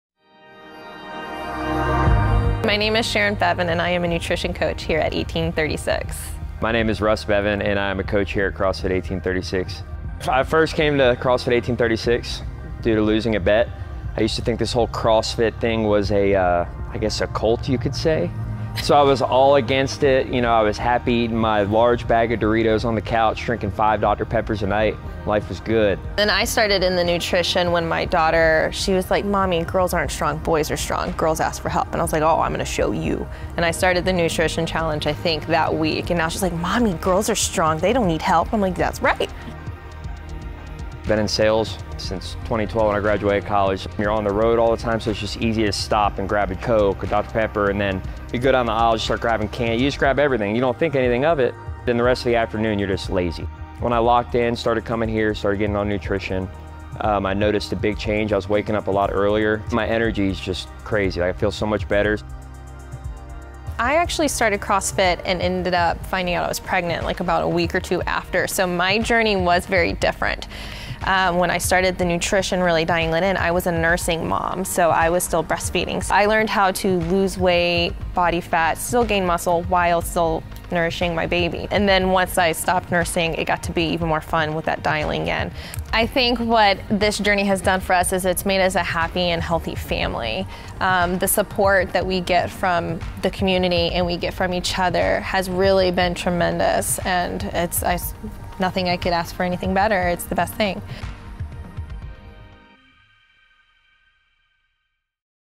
Meet Russ & Sharon, Coach and Nutrition Coach at CrossFit 1836
Russ and Sharon dialed in their nutrition and strength as CrossFit members and nutrition clients at CrossFit 1836. This transformative journey has made them a happy and healthy family.

Russ is now a CrossFit coach and Sharon is a nutrition coach CrossFit 1836.